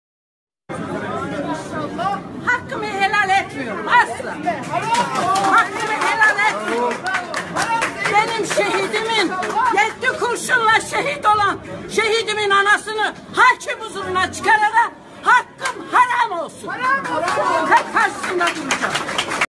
Pakize Akbaba: "Beni hakim karşısına çıkaranlara hakkımı helal etmiyorum!"
Şehit Anaları Derneği Başkanı Pakize Alp Akbaba: "Hakkımı helal etmiyorum. Asla. Benim oğlumun(Şehidimin) üzerinden 7 kurşun çıktı, beni hakim karşısına çıkaranlara hakkımı helal etmiyorum."